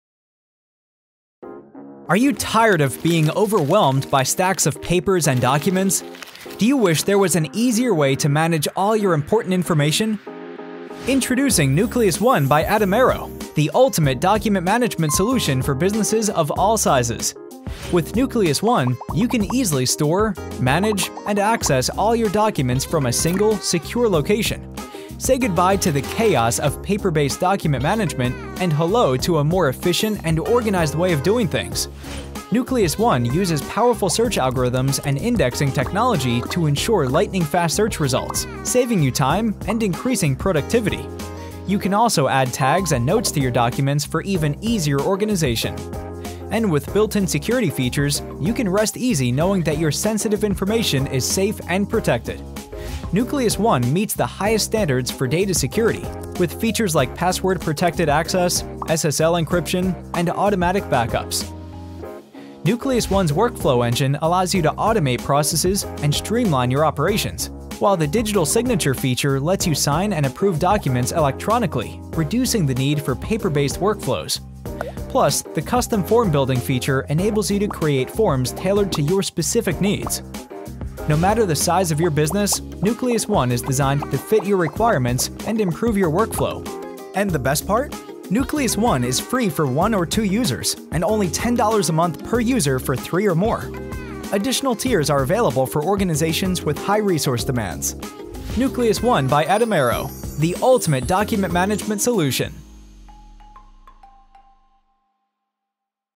Nucleus One | The Ultimate Document Management Solution
Nucleus One is the perfect way to manage documents and important information for any organization. You can greatly reduce your need for paper-based work with features like form building, digital signatures, and custom workflows.

Every feature of Nucleus One is free for one or two users. Try it today at nucleus.one